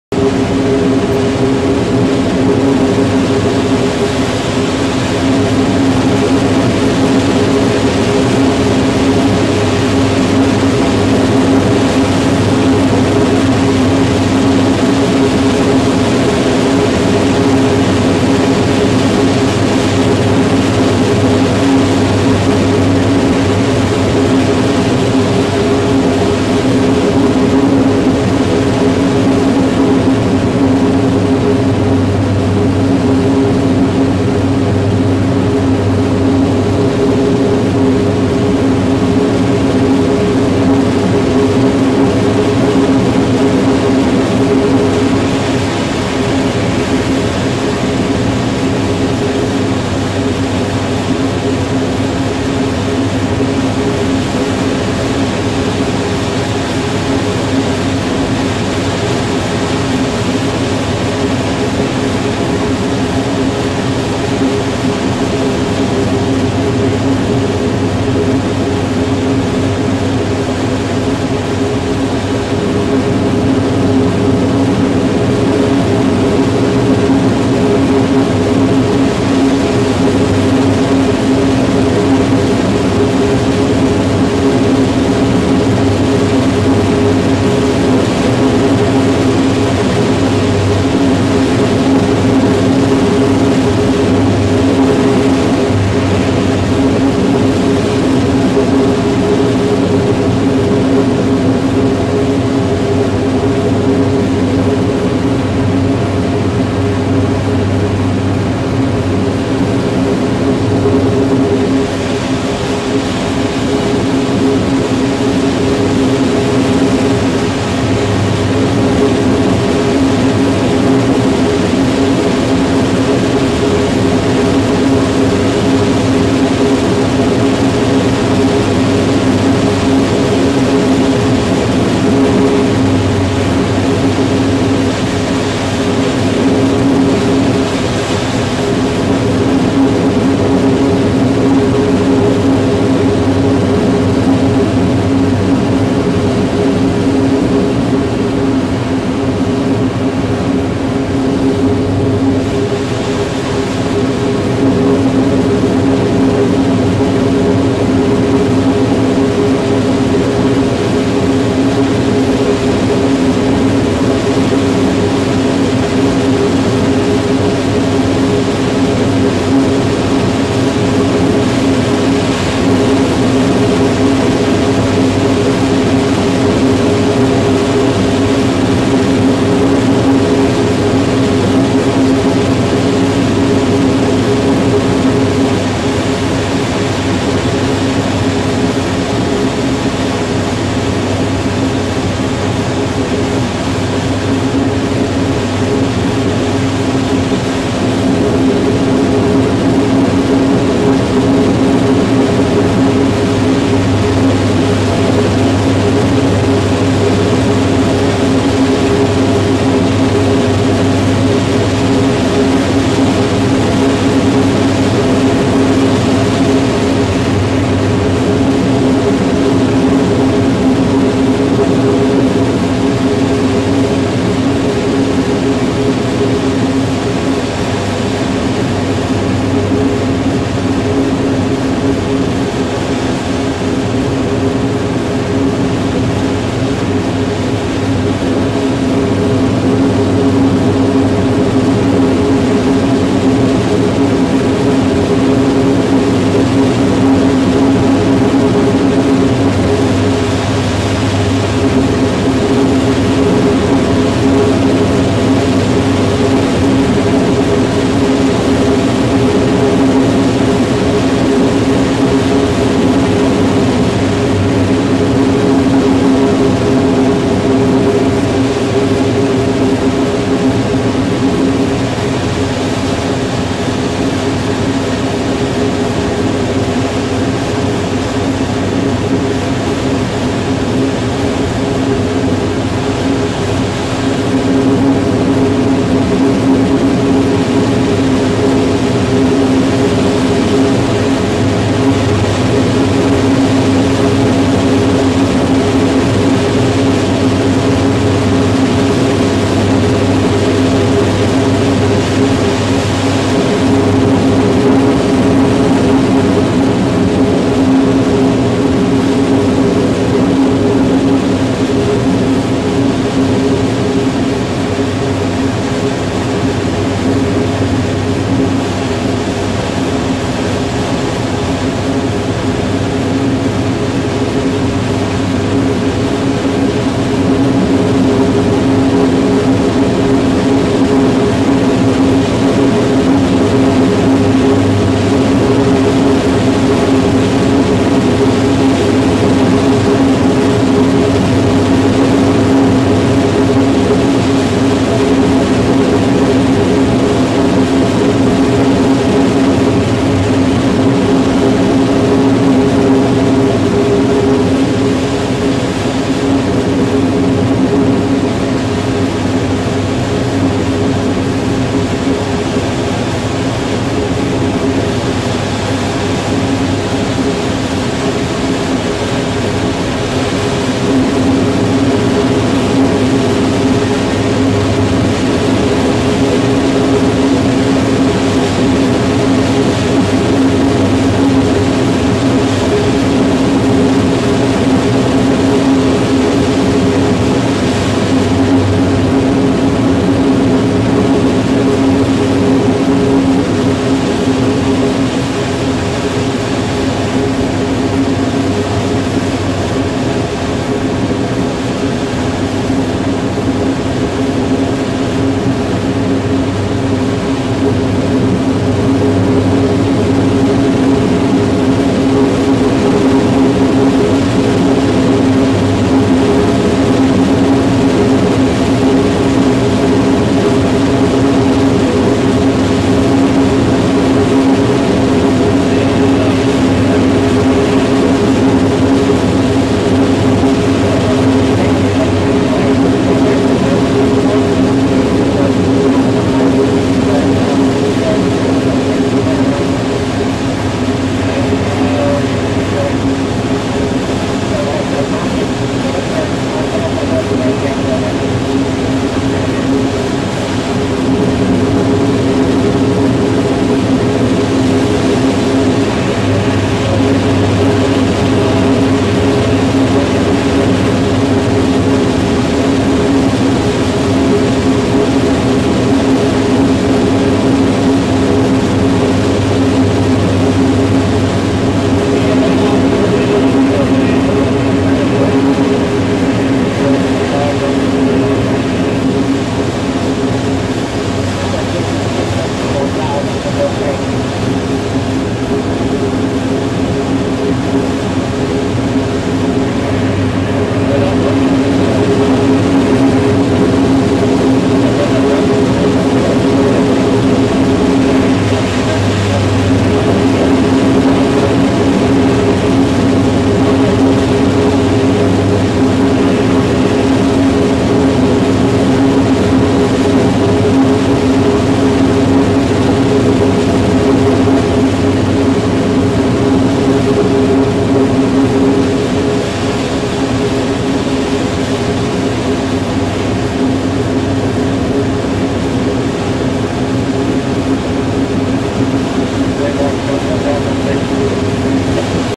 Crop Duster Negotiating Power Lines
Unedited footage of crop dusting cotton in a field with a high-line. The pole at 5:00 couldn't be much more in the way. Crop Dusters have to deal with obstacles like these on a daily basis. New edited videos on the way too.